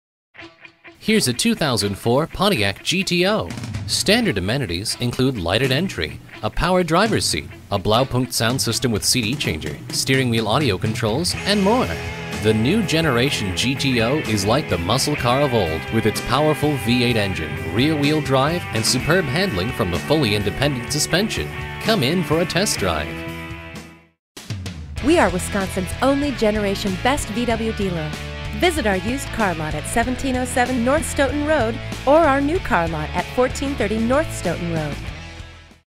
2004 Pontiac GTO #95931 in Madison WI Middleton, WI 53714 - SOLD
SOLD - If you are looking for real value on a great used car, Zimbrick Volkswagen of Madison invites you to come in and test drive this 2004 Pontiac GTO, stock# 95931. We are conveniently located near Madison WI Middleton, WI and known for our great selection, reliability and quality. Come take a look at this 2004 Pontiac GTO today.

Zimbrick Volkswagen of Madison
1430 North Stoughton Road
Madison WI Middleton WI, 53714